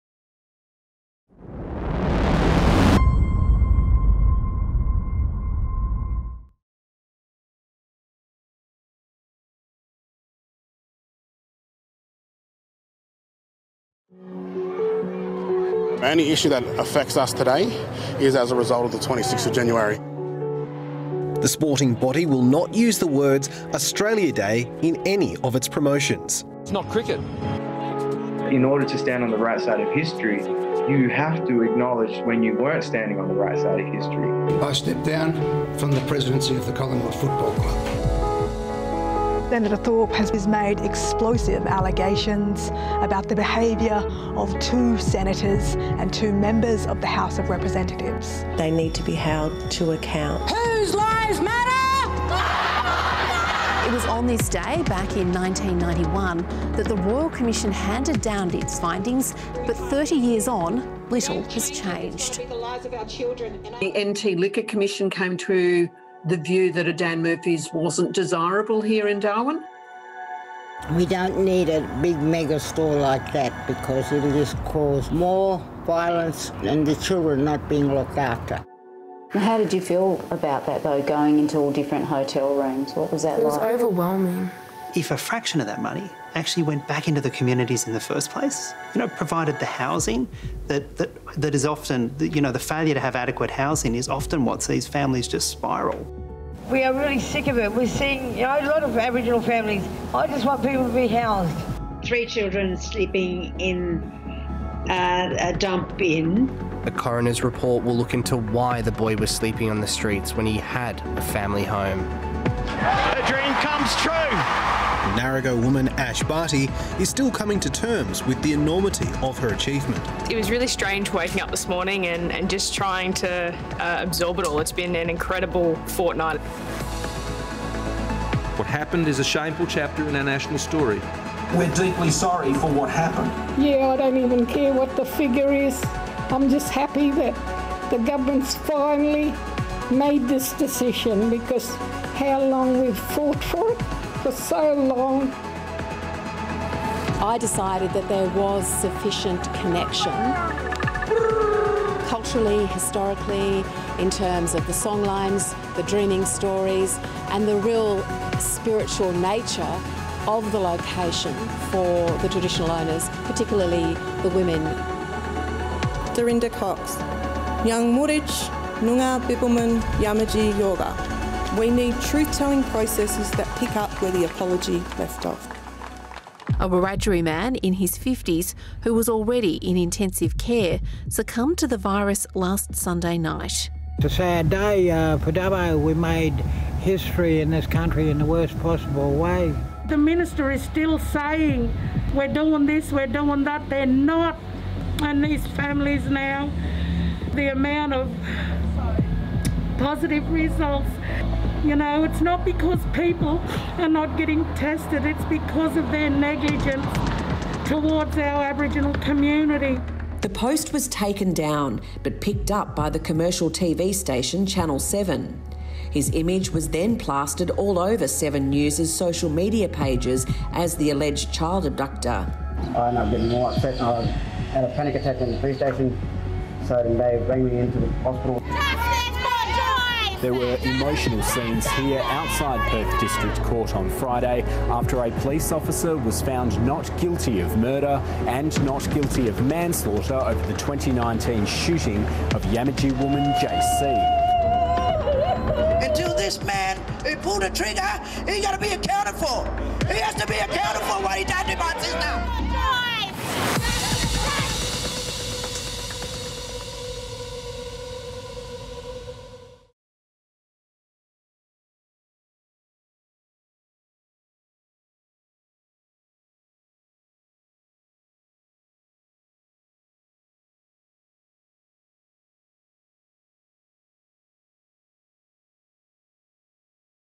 2021: The year in Indigenous affairs | SBS News
2021 was an enormous year for Indigenous affairs, with many First Nations communities severely impacted. But there was a lot to celebrate in the year that was.

This video contains the voices and images of people that have passed. Some viewers may find some of the footage upsetting.